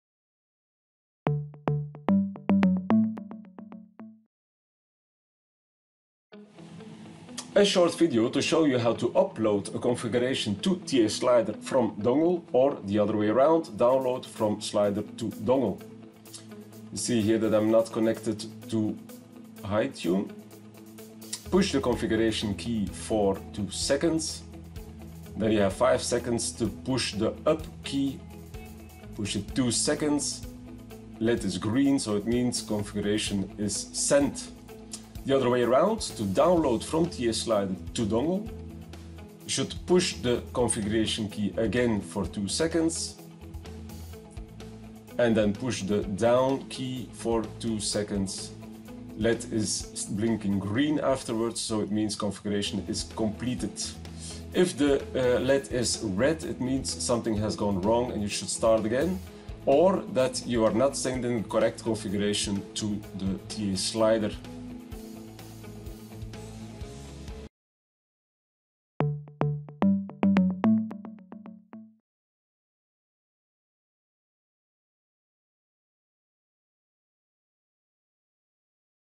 TA Slider - Dongle _ Configuration Upload/Download
With TA Dongle you have the possibility to send the configuration directly to TA Slider, without connecting to Hytune. This could be a configuration already prepared in the offline configuration mode, or downloaded from a similar TA Slider. This is useful for configuring multiple TA Sliders in a project with the same settings.
A configuration should always be made for the specific type of Slider, for example uploading TA Slider 160 Plus to a TA Slider 500 won't work.